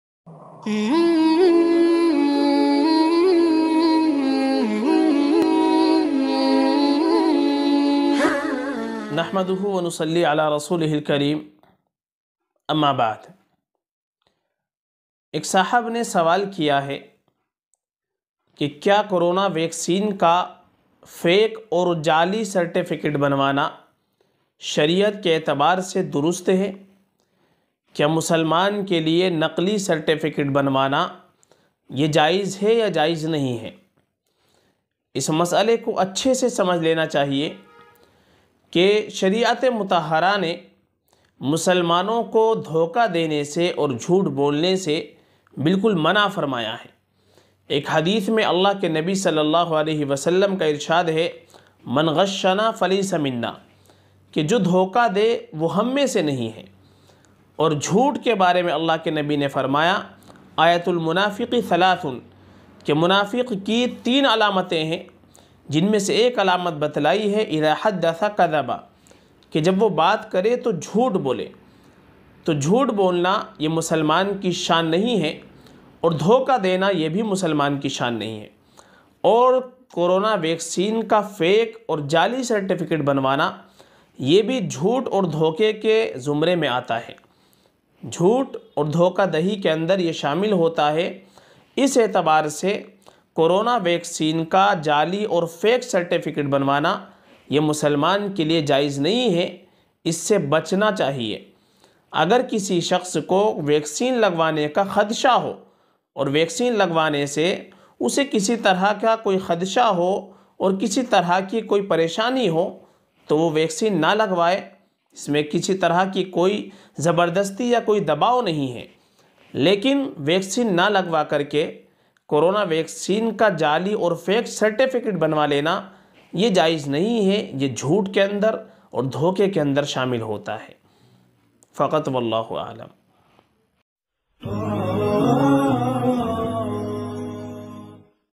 Corona vaccine ka fake certificate banwana kaisa hai?(important clip) by Mufti Sadaqat Husain qasmi
Kya corona vaccine ka fake ( nakli, jali) certificate banana jayiz hai? Vaccine lagwane bagair certificate banwana kaisa hai? By Mufti Sadaqat Husain qasmi

کورونا ویکسین کا جعلی سرٹیفکیٹ بنوانا کیسا ہے، ویکسین لگوائے بغیر نقلی سرٹیفکیٹ بنوانا مسلمان کے لیے جائز ہے یا نہیں؟ مفتی صداقت حسین قاسمی